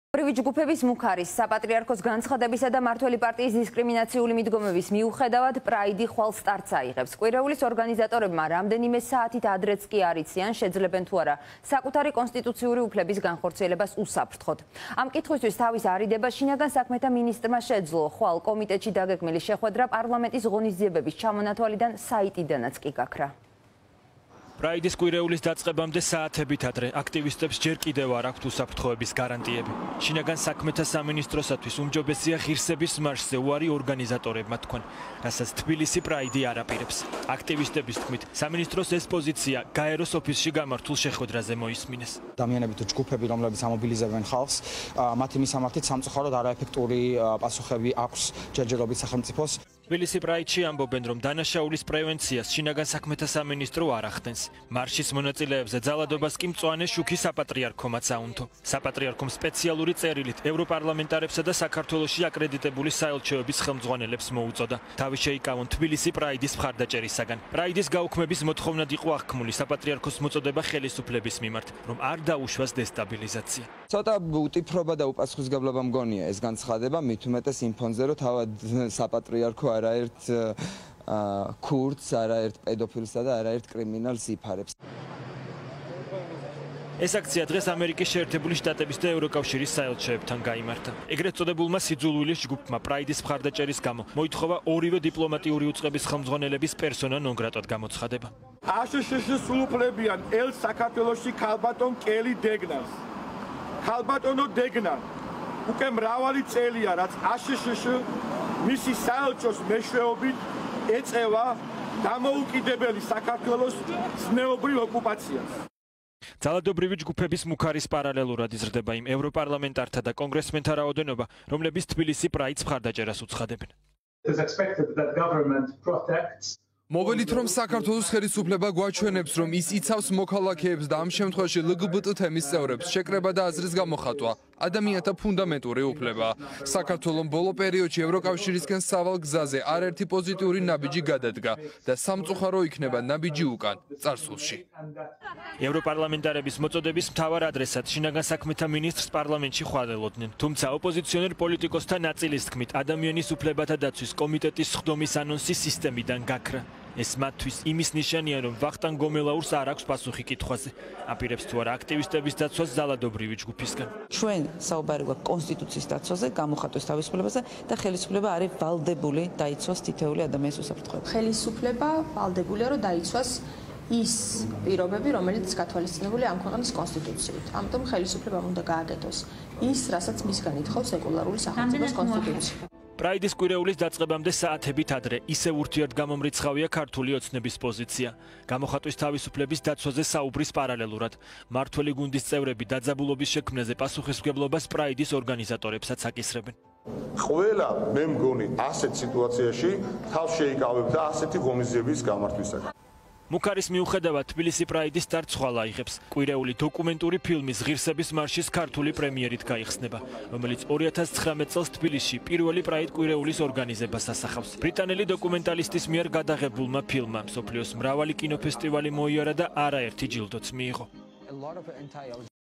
თბილისი პრაიდი უსაფრთხორბის გარანტიის გარეშე
▪️ ძალადობრივი ჯგუფების მუქარის, საპატრიარქოს განცხადებისა და მმართველი პარტიის დისკრიმინაციული მიდგომების მიუხედავად, პრაიდი ხვალ სტარტს აიღებს - აქტივისტებს ჯერ კიდევ არ აქვთ უსაფრთხოების გარანტიები